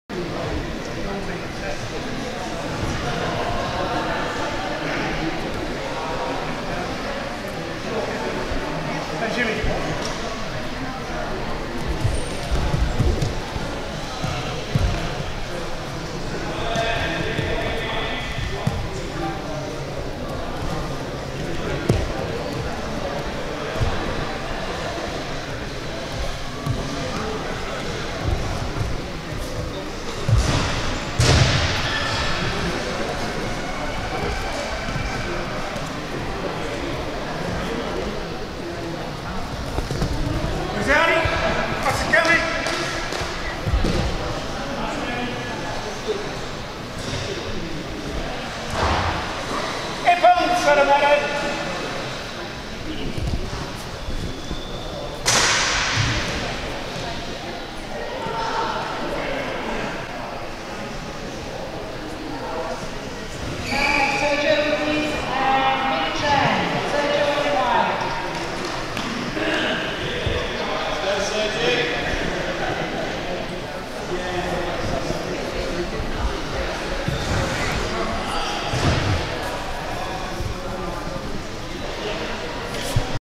2020 Autumn Championships, Contest 5, Senior Women u52kg, Amy Trinh Suzanne Wells
2020 Judo SA Autumn Championships